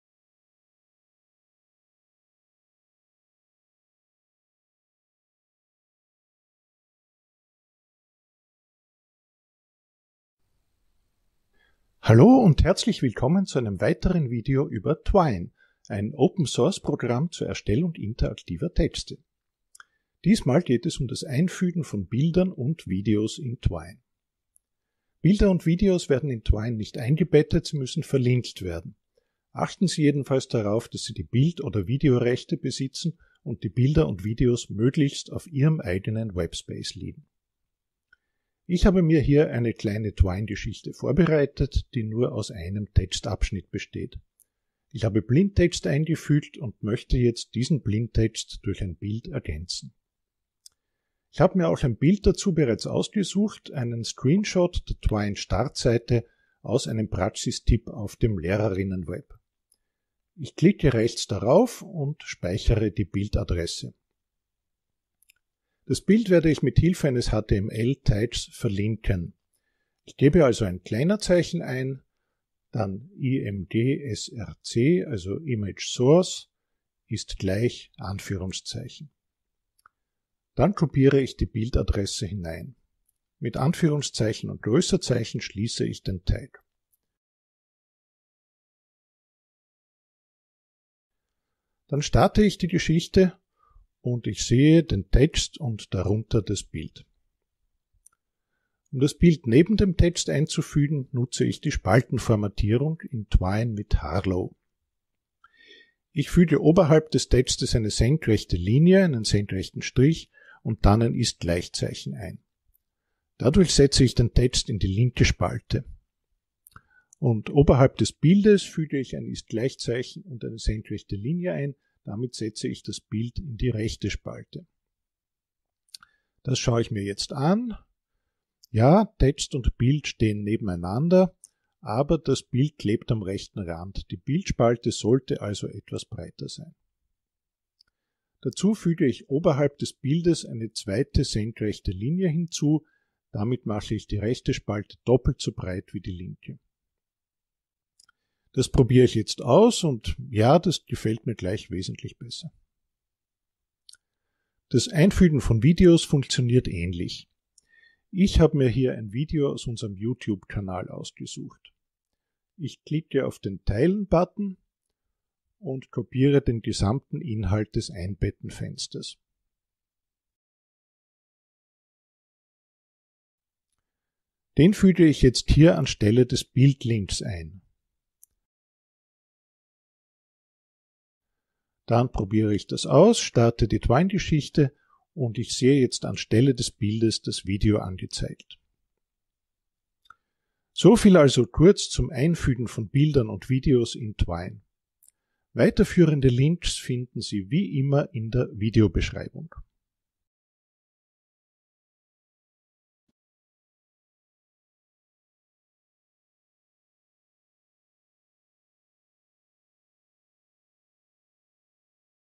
Twine Teil 4: Bilder und Videos einfügen
Bilder und Videos werden in Twine nicht eingebettet, sondern müssen verlinkt werden. In diesem Video zeigen wir, wie das im Standard-Storyformat Harlowe funktioniert und wie Text und Bild in getrennten Spalten angezeigt werden können.